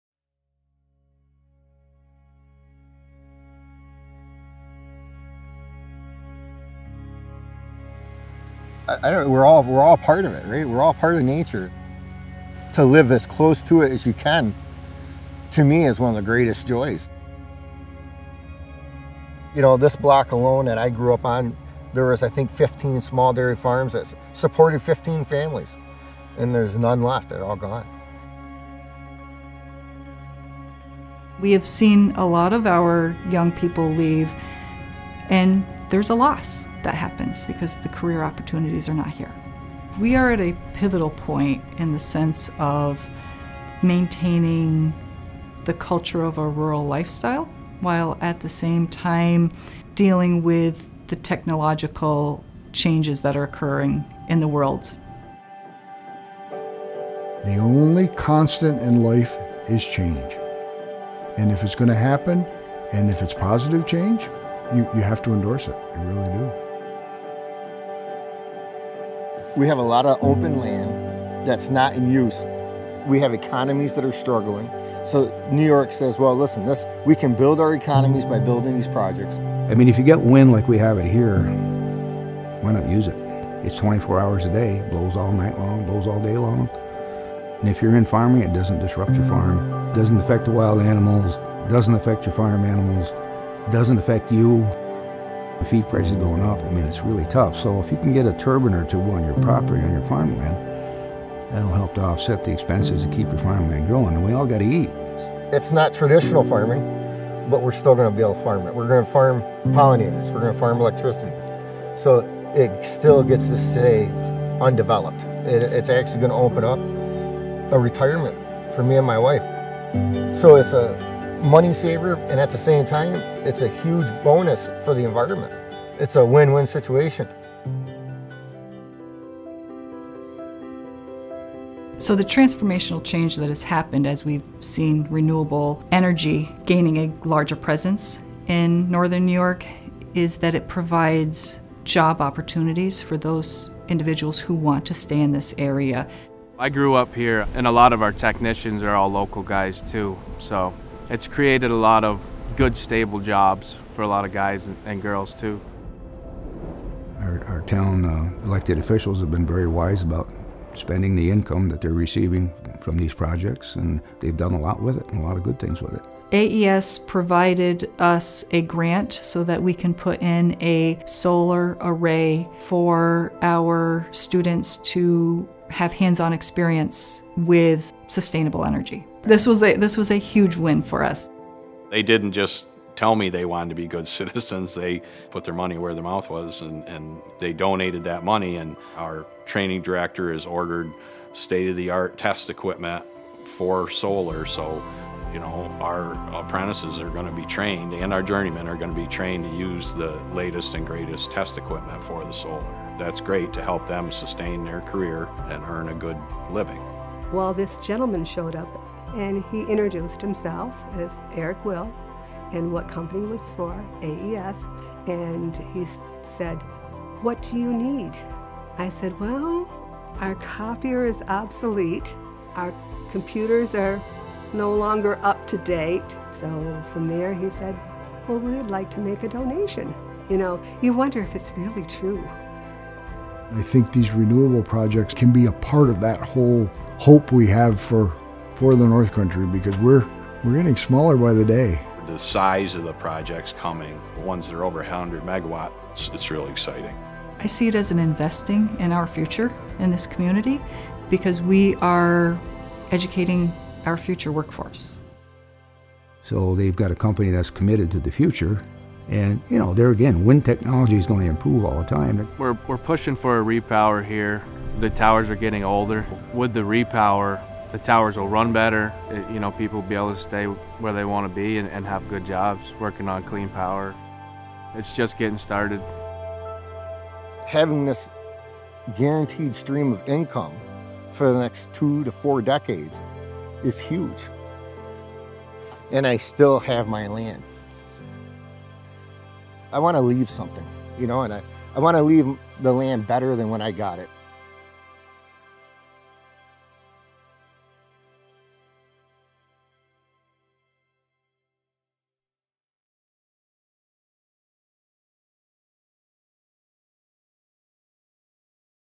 AES supports New York's clean energy transition
New York is a leader in the clean energy transition with some of the most ambitious renewable targets in the nation to reach 70% renewables by 2030. With more than two decades of experience working in the state, AES has helped New York lead the adoption of solar, solar-plus-storage, and standalone storage at the community and utility-scale.

From Upstate to Downstate and everywhere in between, we utilize our experience and in-house expertise to foster partnerships with customers and co-create solutions to responsibly meet the energy and economic needs of communities across the state. Whether partnering with communities, utility customers, or Fortune 500 companies, we make achieving New York's clean energy targets beneficial for everyone.